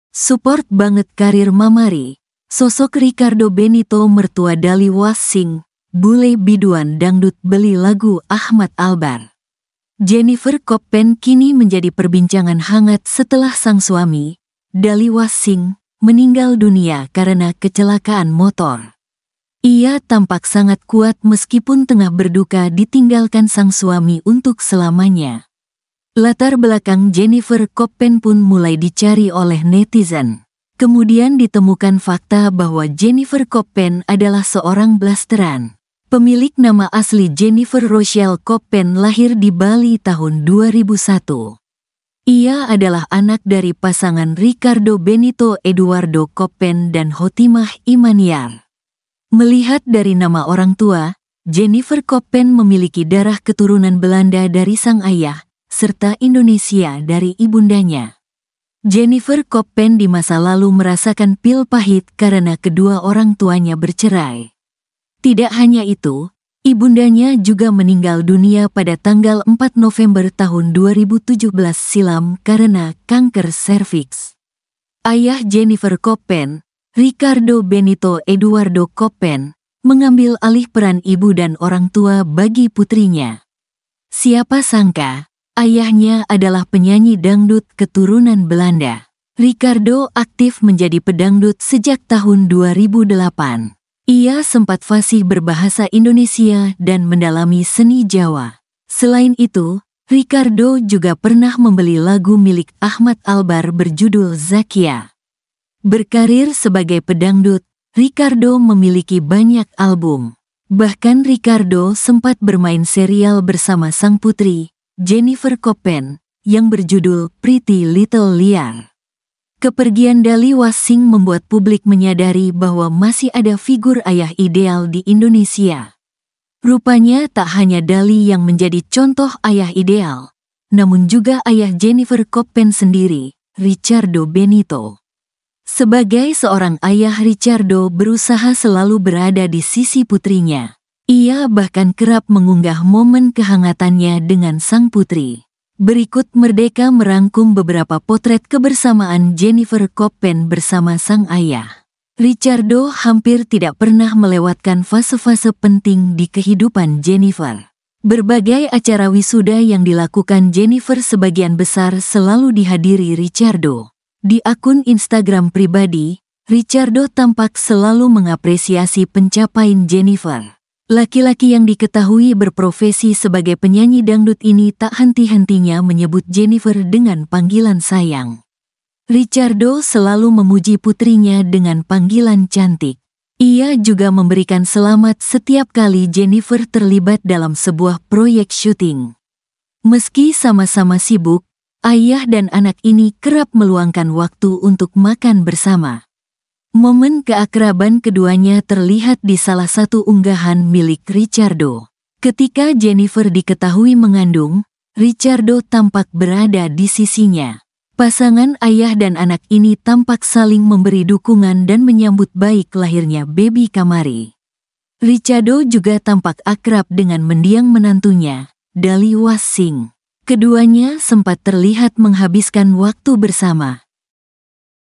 SOSOK RICARDO BENITO MERTUA DALI WASSINK BULE BIDUAN DANGDUT BELI LAGU AHMAD ALBAR
"Support Banget Karir Mamari," Sosok Ricardo Benito Mertua Dali Wassink, Bule Biduan Dangdut Beli Lagu Ahmad Albar.

Jennifer Coppen kini menjadi perbincangan hangat setelah sang suami, Dali Wassink, meninggal dunia karena kecelakaan motor. Ia tampak sangat kuat meskipun tengah berduka ditinggalkan sang suami untuk selamanya. Latar belakang Jennifer Coppen pun mulai dicari oleh netizen. Kemudian ditemukan fakta bahwa Jennifer Coppen adalah seorang blasteran.

Pemilik nama asli Jennifer Rochelle Coppen lahir di Bali tahun 2001. Ia adalah anak dari pasangan Ricardo Benito Eduwardo Coppen dan Hotimah Imaniar. Melihat dari nama orang tua, Jennifer Coppen memiliki darah keturunan Belanda dari sang ayah, serta Indonesia dari ibundanya.